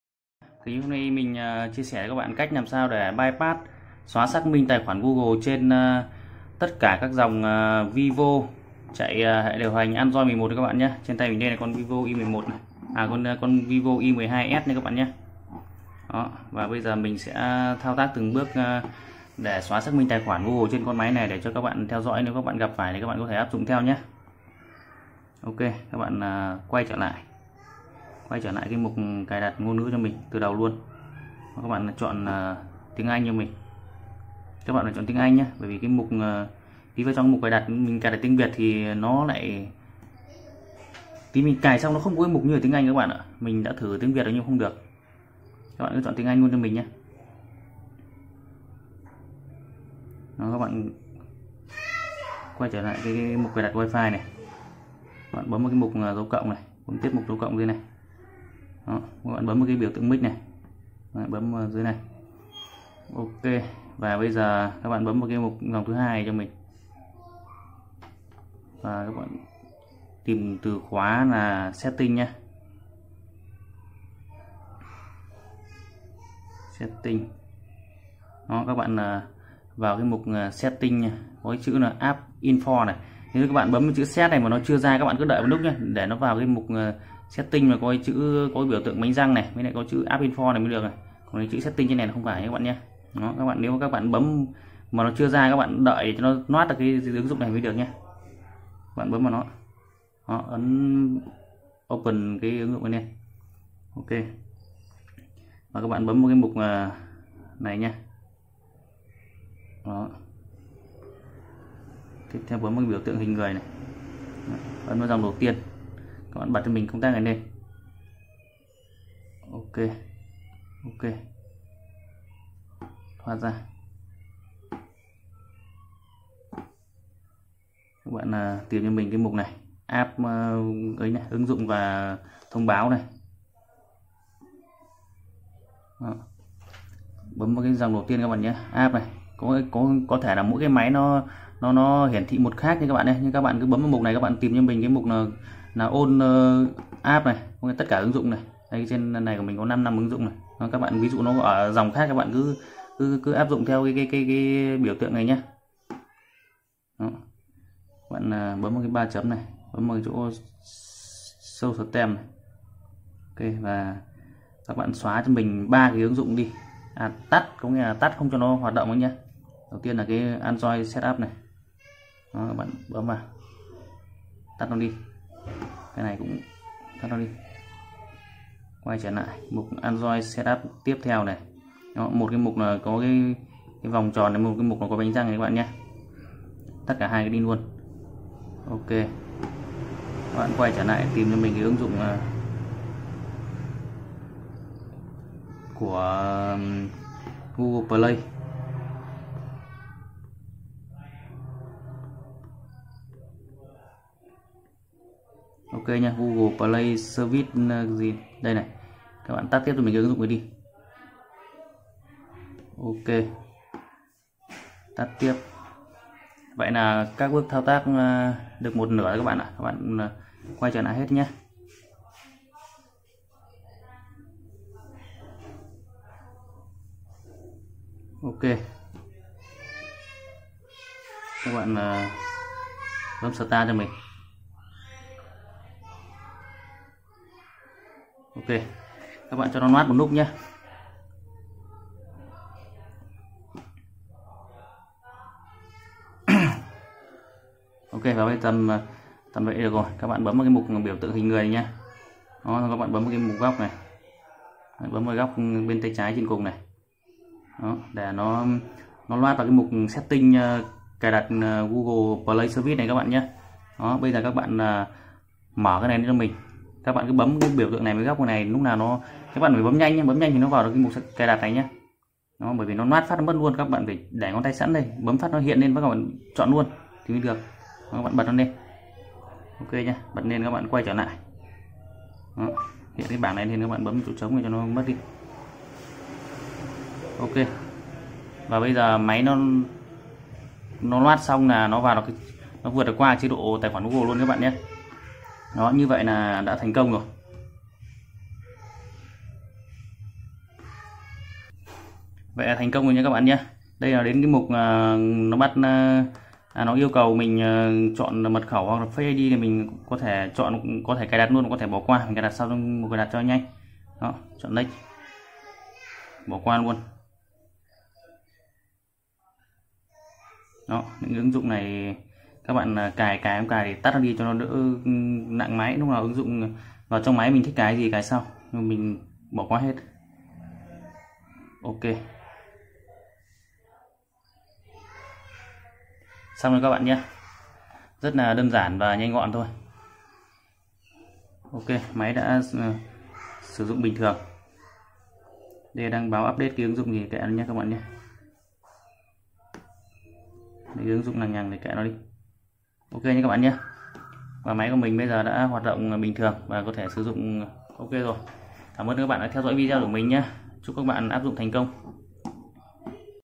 Xóa Xác Minh Tài Khoản Google ViVo Y12s Và Tất Cả ANDROID 11 Mới Nhất 2021
- clip hôm nay mình chia sẻ cho cách  Frp Bypass Android 11 Google Account Lock latest Method 2021
- các bạn gặp khó khăn ở bước nào để lại commen mình sẽ giải đáp giúp các bạn. chúc các bạn thành công !
bypassvivoandroid11 bypassnew2021